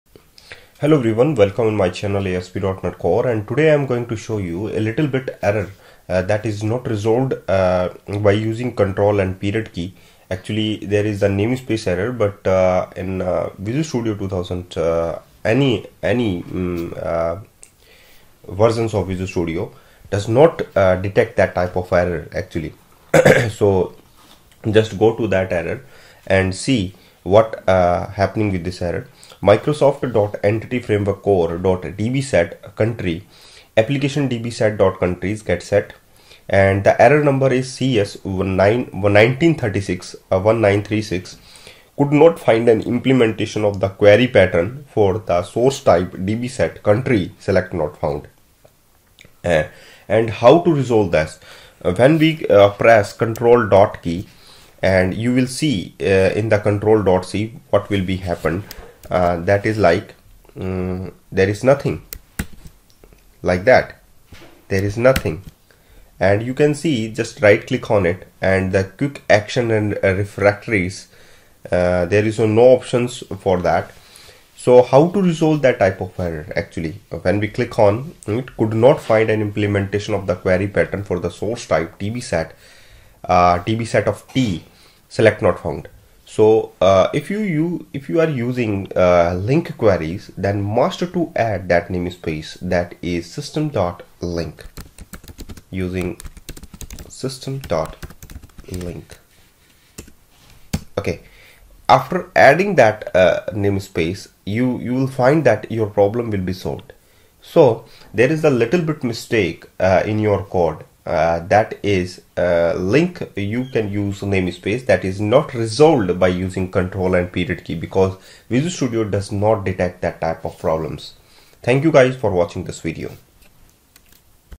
Fix : Could not find an implementation of the query pattern for source type DbSet Select not Found
In this video, I am going to show you how to fix query pattern for source type.